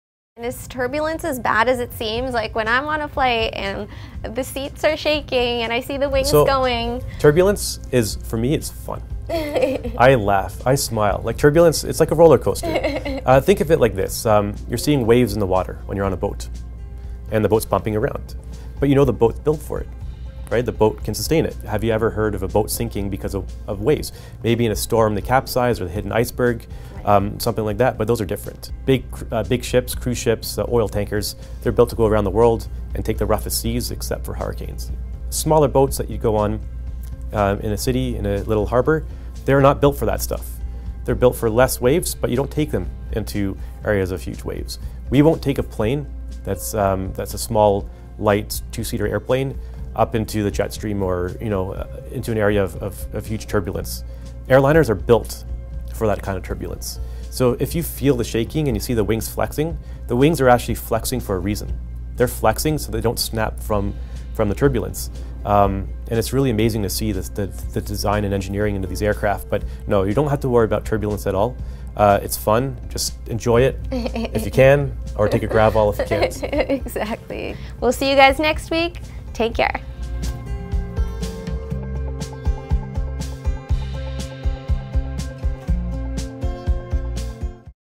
Airplane Turbulence Explained by a Pilot (WHO KNEW?!)
Airplane Turbulence Explained by a Pilot (WHO KNEW?!)//Ever had a bumpy flight? Turbulence and weather have a role to play in that. Do you wonder what airplane turbulence is and why it’s no big deal? When is turbulence dangerous? It can be scary to watch someone say, here is some turbulence caught on camera. Thunderstorms and severe turbulence are unavoidable. But does turbulence cause planes to crash? No. I’m sure the life of a flight attendant and crew would be interesting to hear about, don’t you think?

Cameras I used:
Sony a6500
iPhone X

Lenses I used for the Sony Camera:
Sony 10-18mm
Song 28-70mm
Sony 50 mm